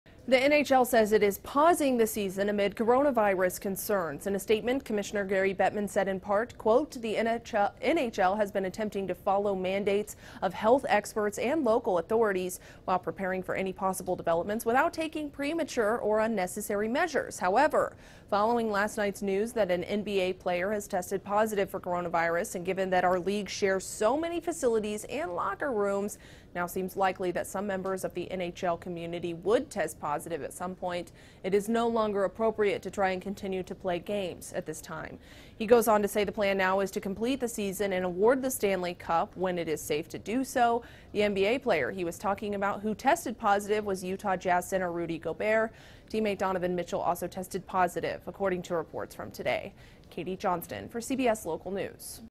NHL Suspends Season Due To Coronavirus Outbreak
The NHL has suspended the season because of Coronavirus concerns. Katie Johnston reports.